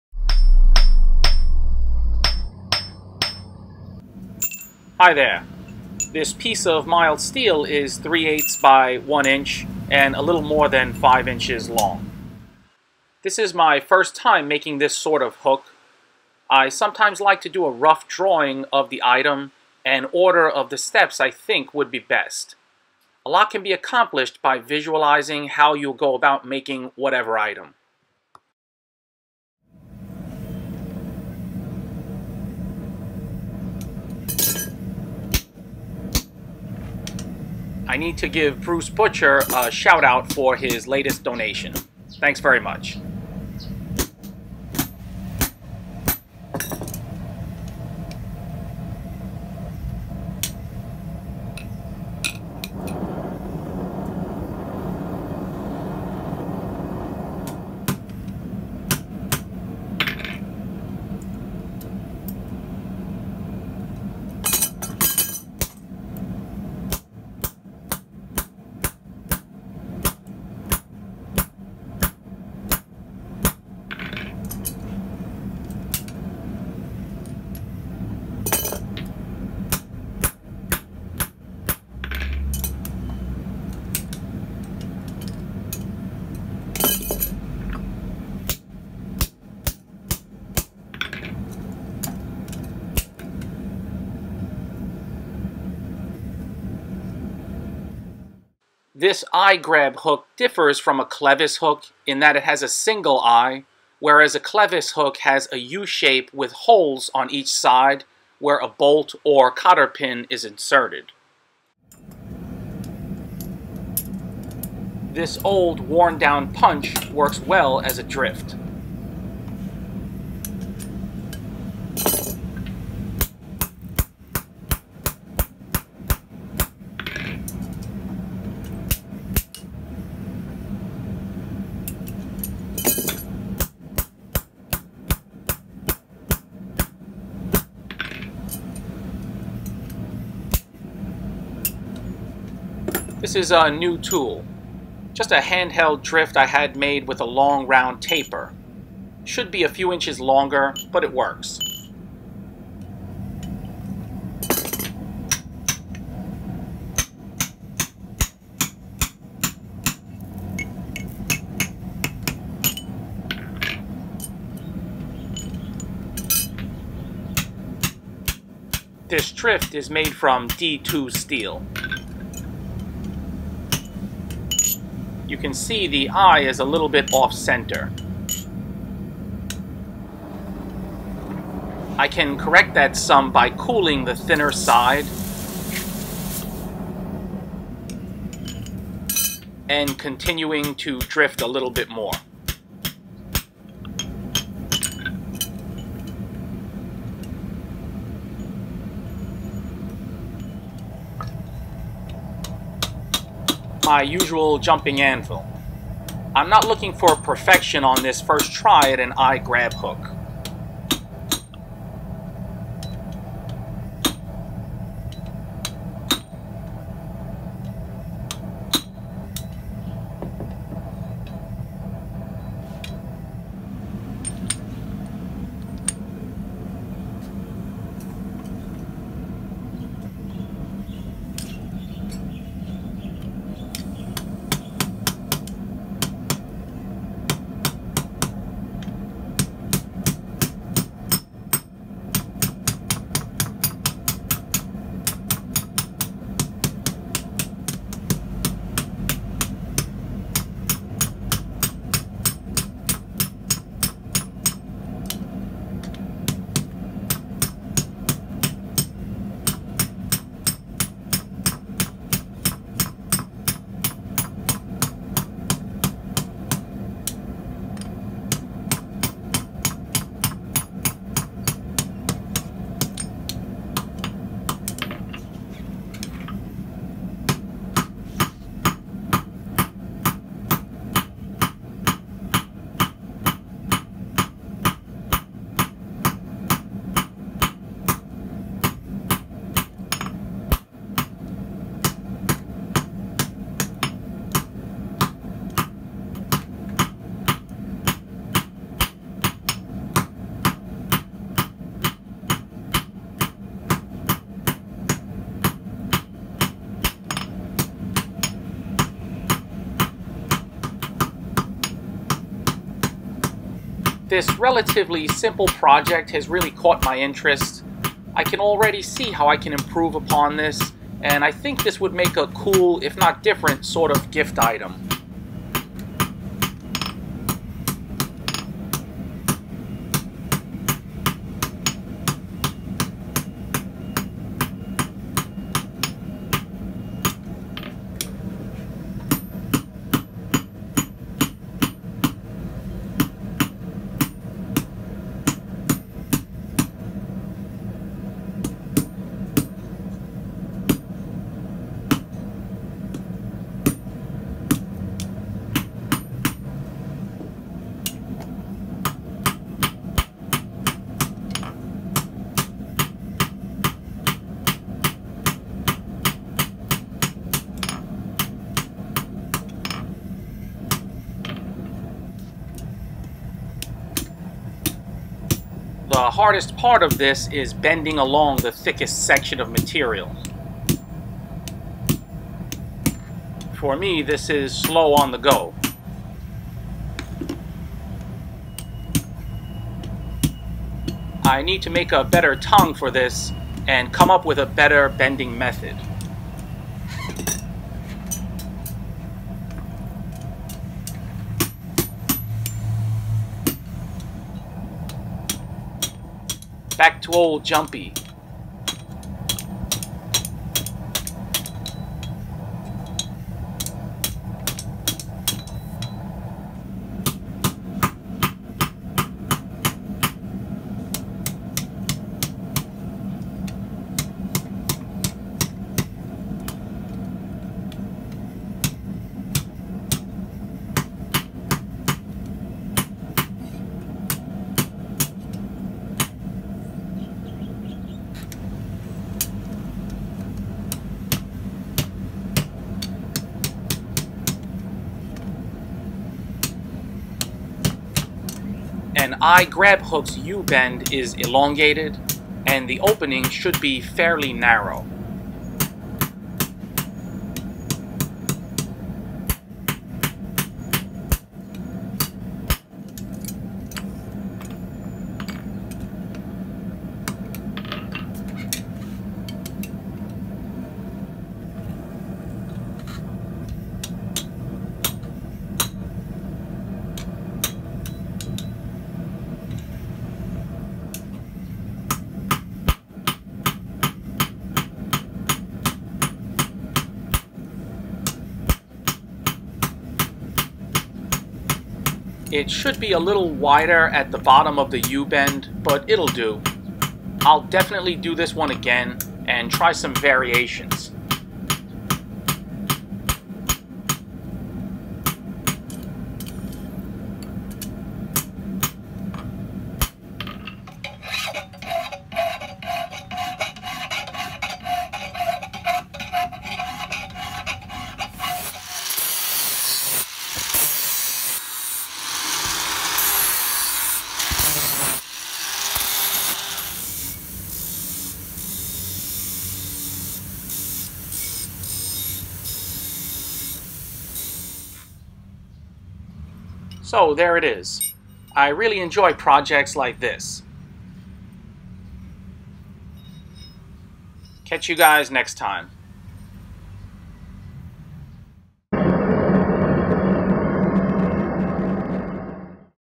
Hand Forged Eye Grab Hook
In this video, I make an eye grab hook for the first time.